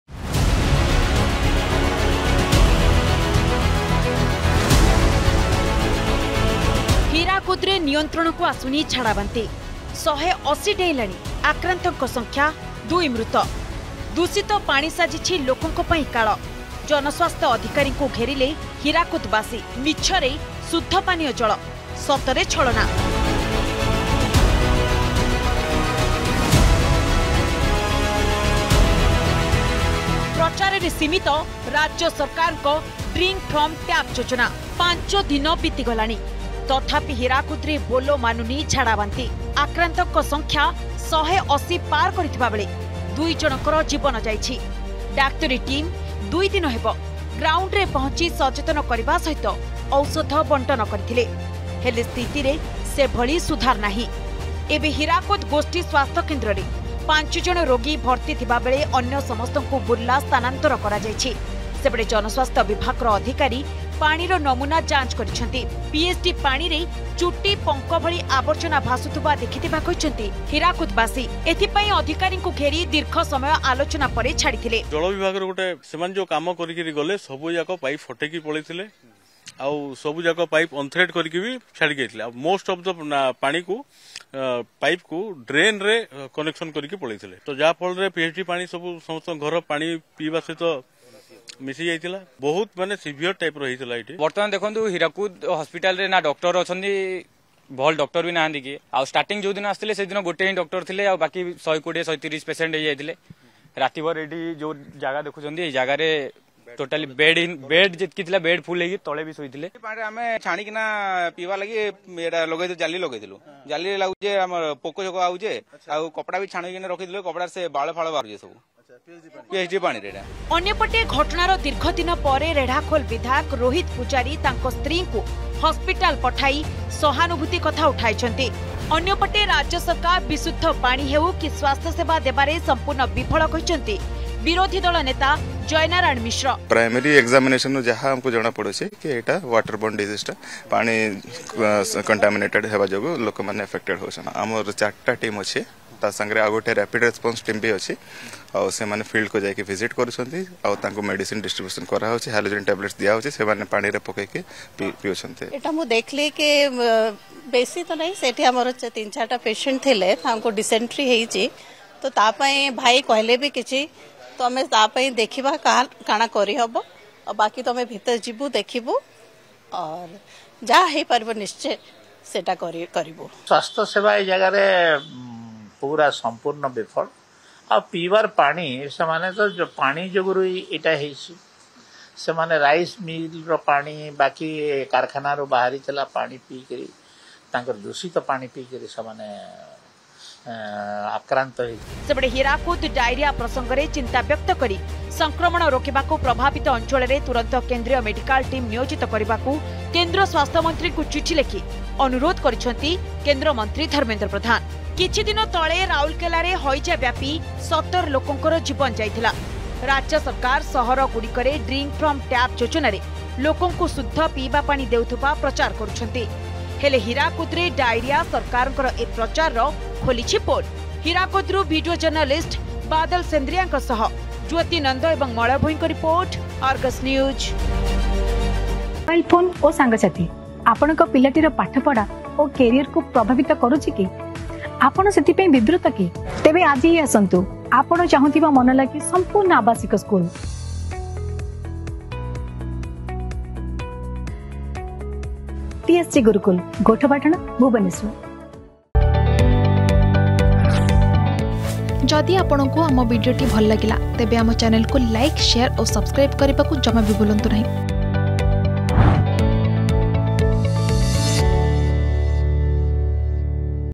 ଦୂଷିତ ପାଣି ସାଜିଛି ଲୋକଙ୍କ ପାଇଁ କାଳ || Health Problem || Hirakud || Odisha Govt
5 ଦିନ ବିତିଗଲା । ତଥାପି ହୀରାକୁଦରେ ବୋଲ ମାନୁନି ଝାଡ଼ାବାନ୍ତି । ଆକ୍ରାନ୍ତଙ୍କ ସଂଖ୍ୟା 180 ପାର୍ କରିଥିଲାବେଳେ 2 ଜଣଙ୍କ ଜୀବନ ଯାଇଛି । ଡାକ୍ତରୀ ଟିମ୍ ୨ ଦିନ ହେବ ଗ୍ରାଉଣ୍ଡରେ ପହଞ୍ଚି ସଚେତନ କରିବା ସହ ଔଷଧ ବଣ୍ଟନ କରିଥିଲେ ।